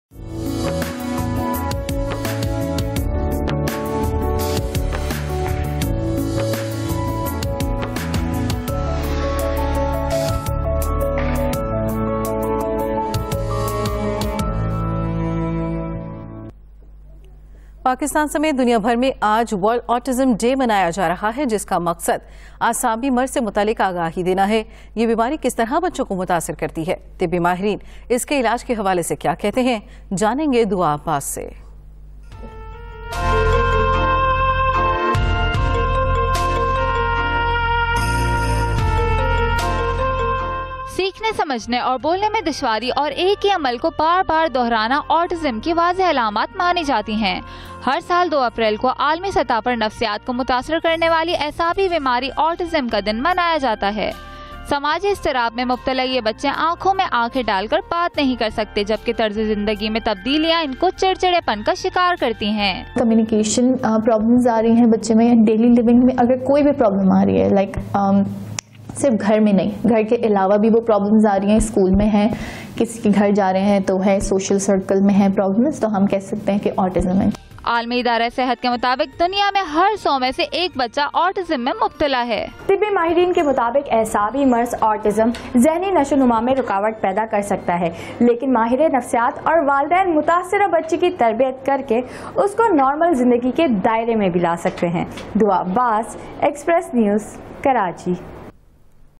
The World Autism Awareness Day Coverage By Express News In DEWA
World Autism Day: Shedding Light on Autism Awareness! 💙

This World Autism Day, DEWA Institute is honored to partner with Express News to raise awareness about autism spectrum disorder (ASD). 🙌

Together, we're dedicated to educating our community about this neurodevelopmental condition and its impact on individuals and families.